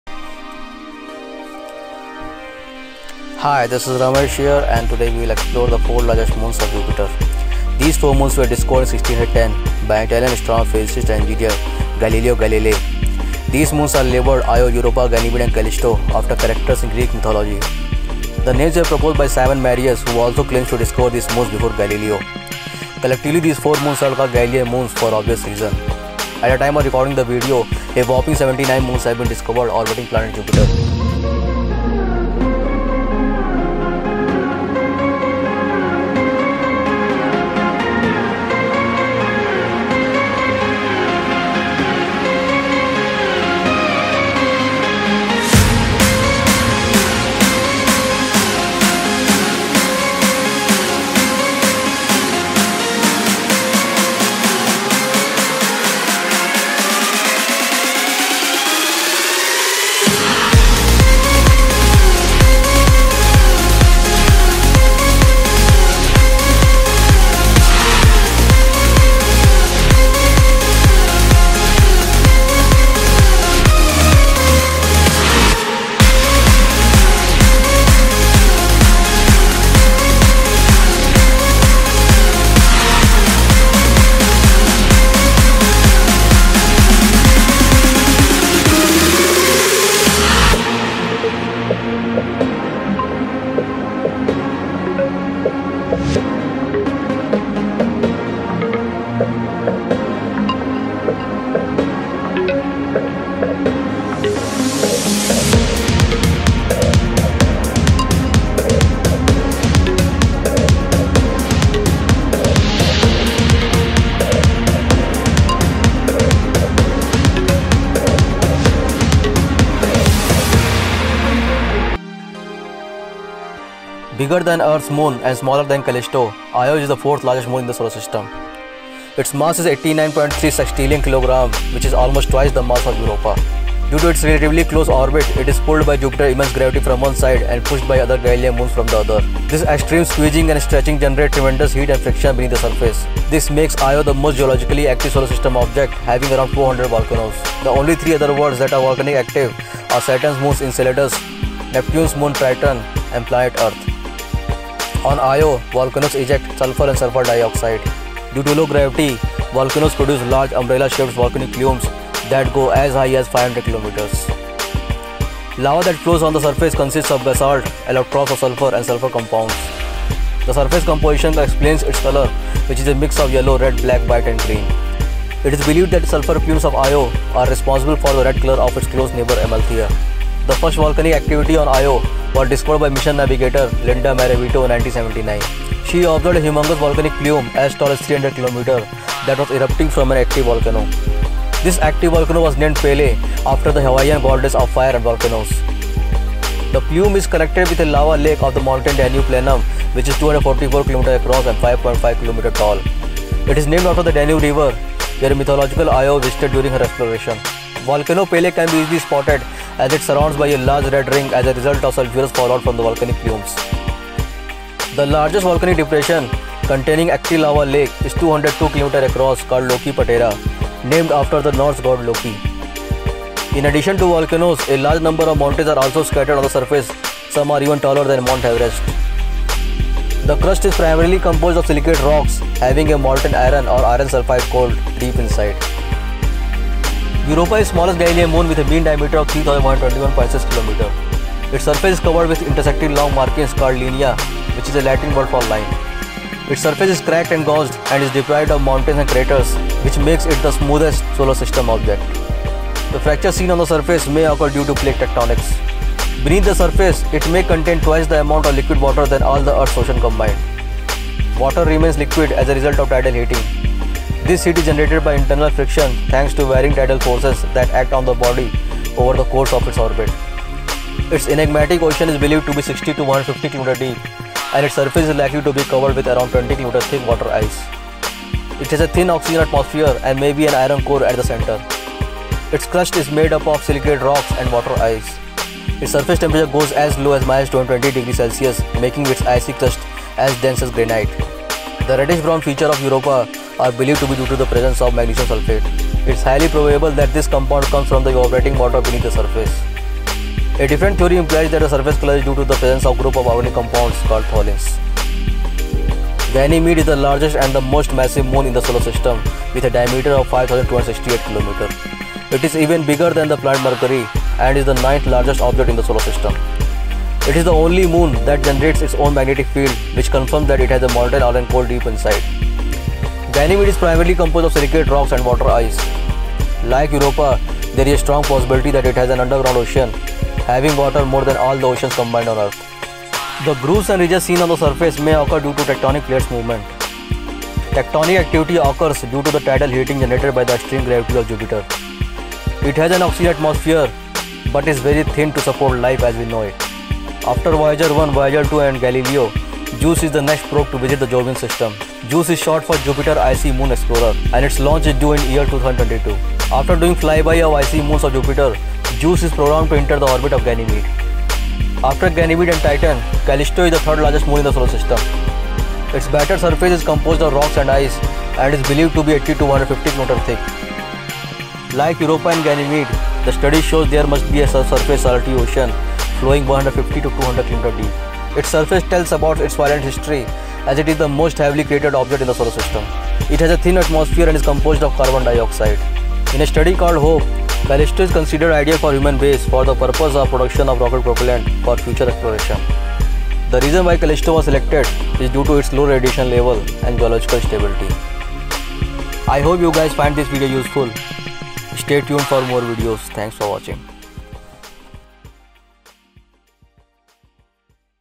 Exploring Io, Europa, Ganymede, and Callisto
In this video, we will explore the four largest moons of Jupiter. These 4 moons were discovered in 1610 by an Italian astronomer, physicist, and engineer Galileo Galilei. These moons are labeled Io, Europa, Ganymede, and Callisto after characters in Greek mythology. At the time of recording the video, a whopping 79 moons have been discovered orbiting the planet Jupiter.

We will also look into the orbital distance of moons of Jupiter and compare their gravity.

Chapters:

0:00 Galilean Moons Intro
0:25 Jupiter's Moons Orbital Distance
1:37 Moons Gravity Simulation
2:01 Io
3:54 Europa
5:05 Ganymede
6:00 Callisto